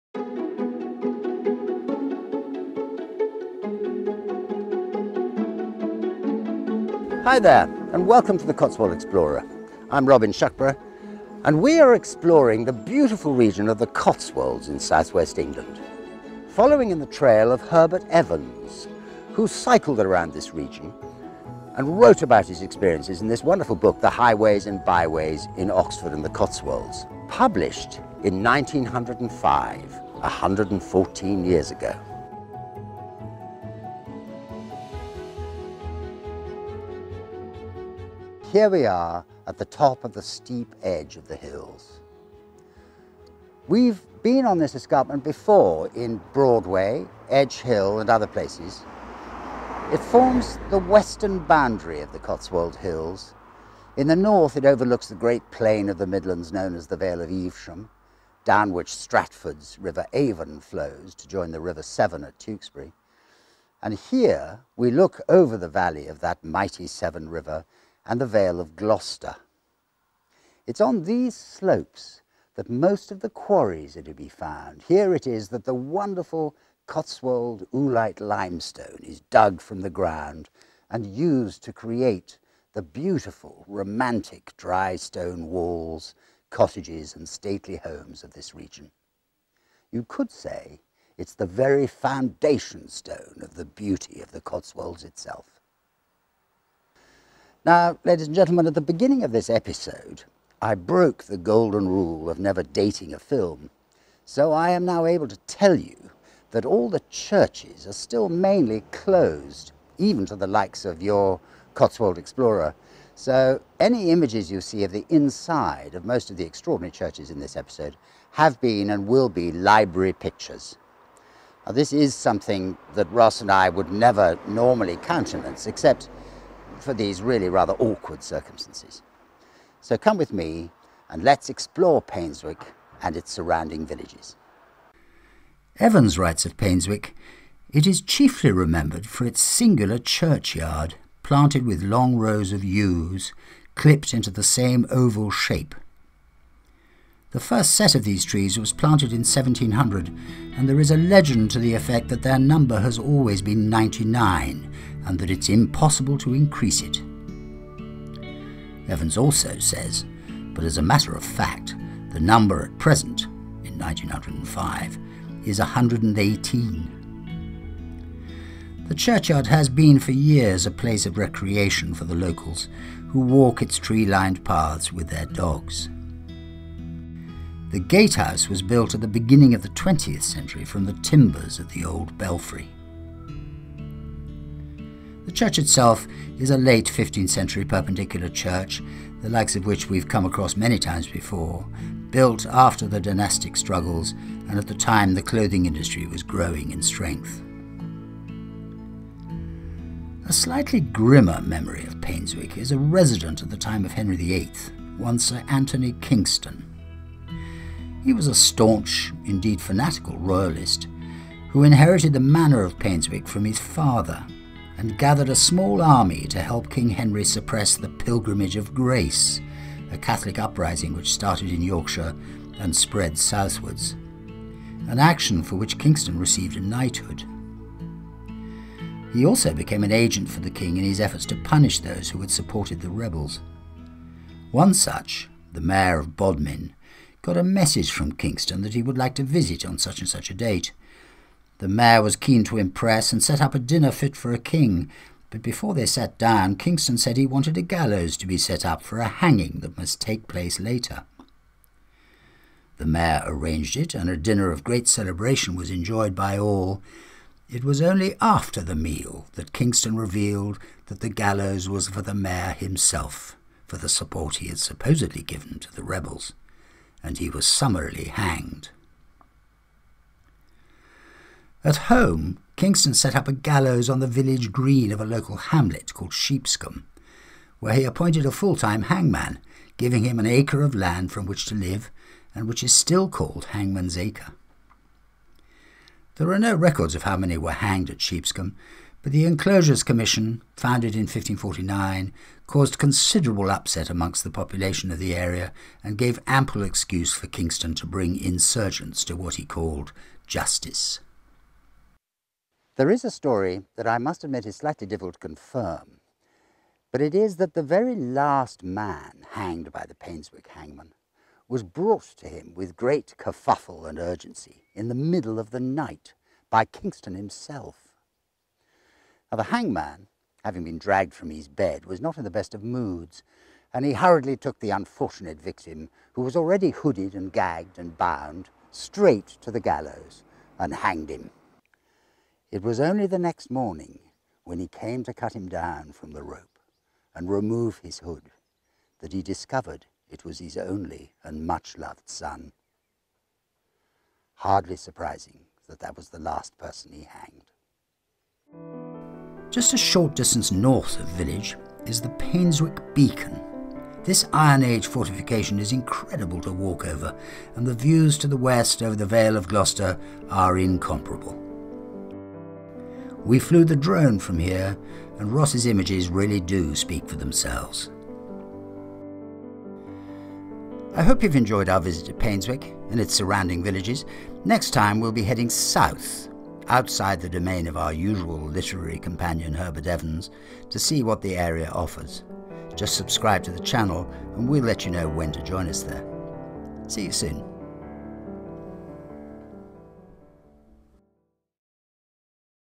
A History of Painswick | Exploring the Cotswolds
Situated hight on the western escarpment of the Cotswolds, Painswick is another classic Cotswold destination in the heart of England.

It’s on these slopes that most of the quarries are to be found. Here it is that the wonderful Cotswold Oolite limestone is dug from the ground and used to create the beautiful romantic dry-stone walls, cottages and stately homes of this region. You could say it’s the very Foundation Stone of the beauty of the Cotswolds itself.